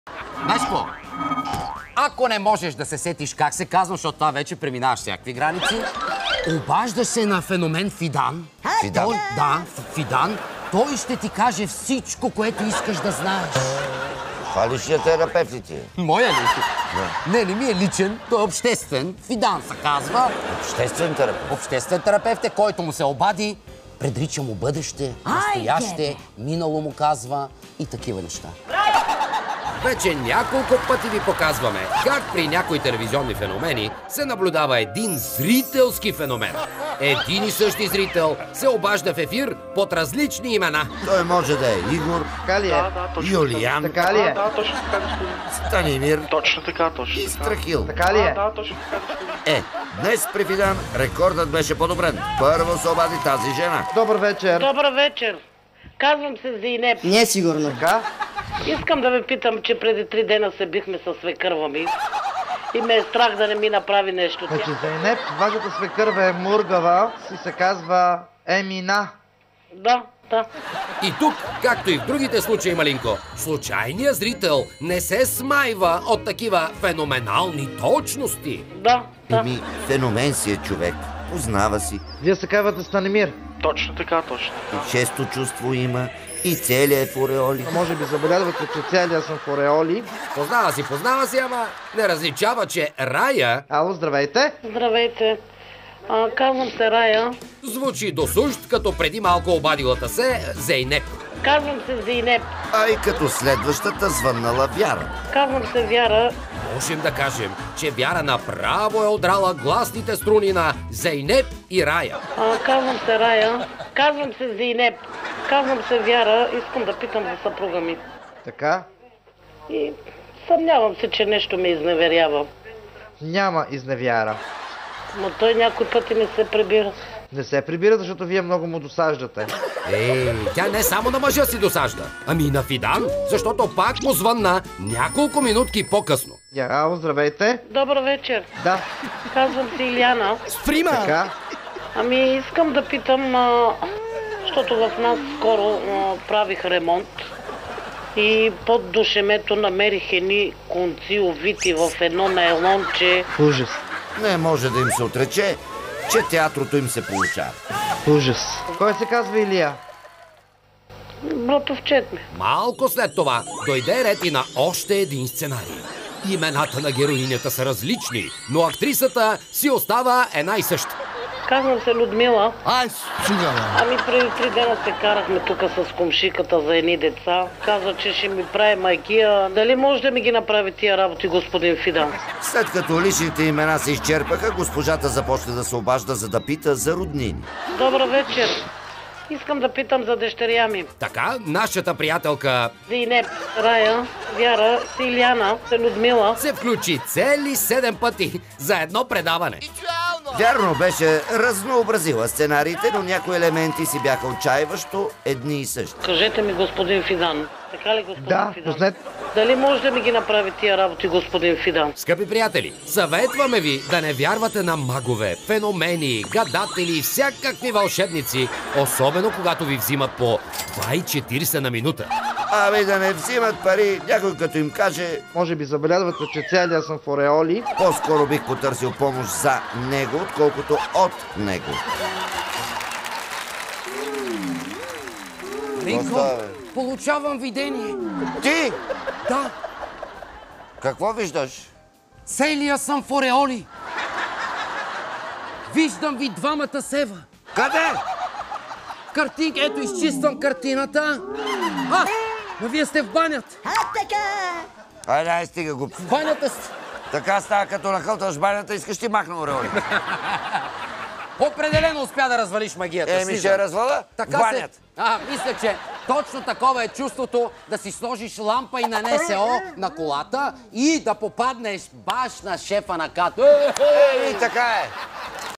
Феноменално нагласени обаждания при маг Фидан
Нагласените обаждания при родни магове с цел зарибяване на още зрители не спират. Вече видяхме няколко такива случая. Един и същи зрител се обажда под различни имена. Днес обаче при маг Фидан рекордните нагласени обаждания бяха подобрени.